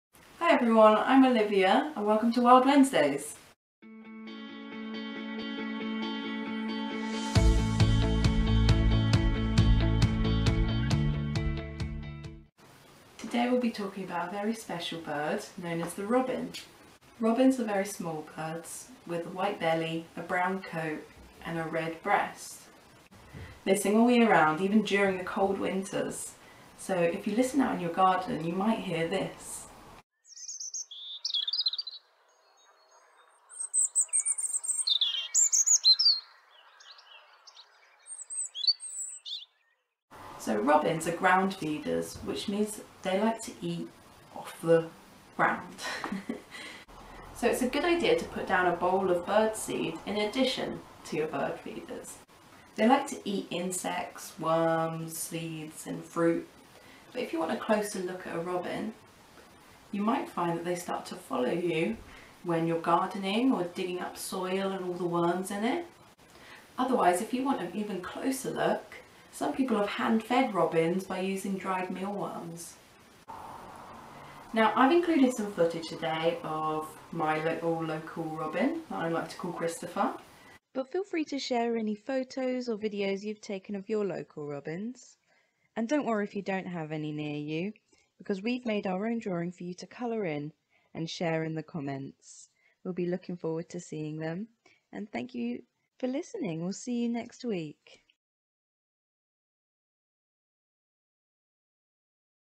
Episode 1: Robins 🐦
It’s a Wild Wednesday in Waltham Forest! And right now we’re getting excited about all things feathered and fabulous 🐥🕊️🦉
Today’s spotlight is on our favourite-festive-feathery-friend (say that 3 times fast)… the Robin!
Olivia's back with some top tips, so let us know what garden birds you’ve spotted from your window or daily walks and we'll share your snaps in our stories… if they manage to sit still long enough that is! 👀📸